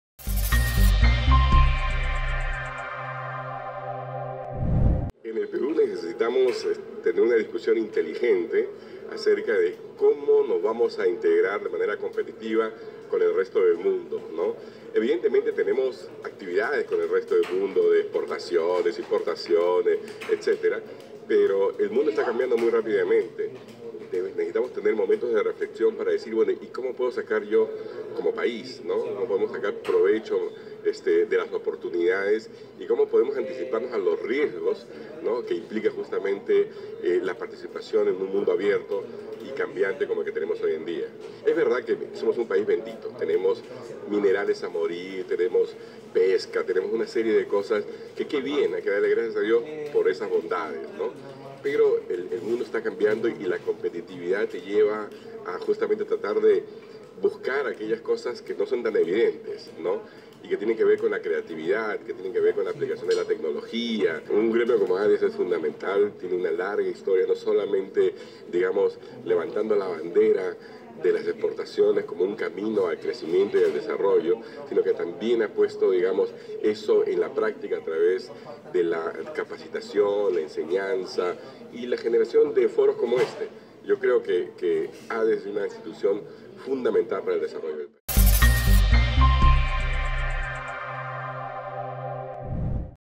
Entrevista a Carlos Anderson
Entrevista a Carlos Anderson durante el I Foro de Políticas de Desarrollo de las Exportaciones.